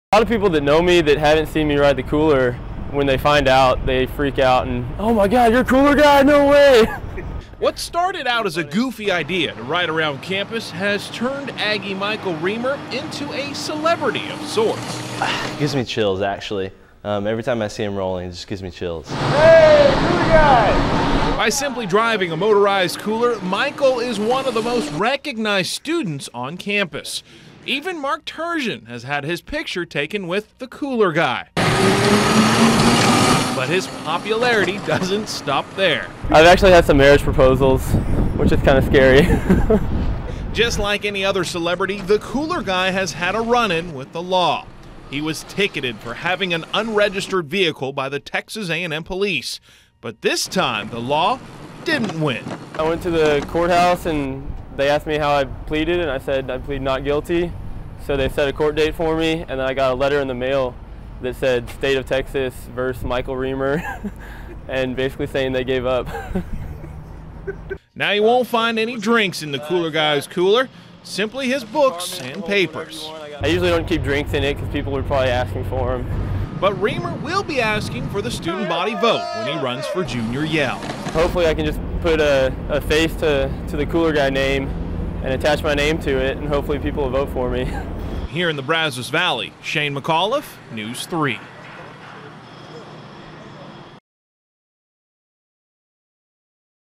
Student's "Cool" Cruiser
Texas A&M students are getting to class just about any way they can, one student rides to class in a motorized cooler. As KBTX's Shane McAuliffe reports, his wheels earned him a special reputation on campus.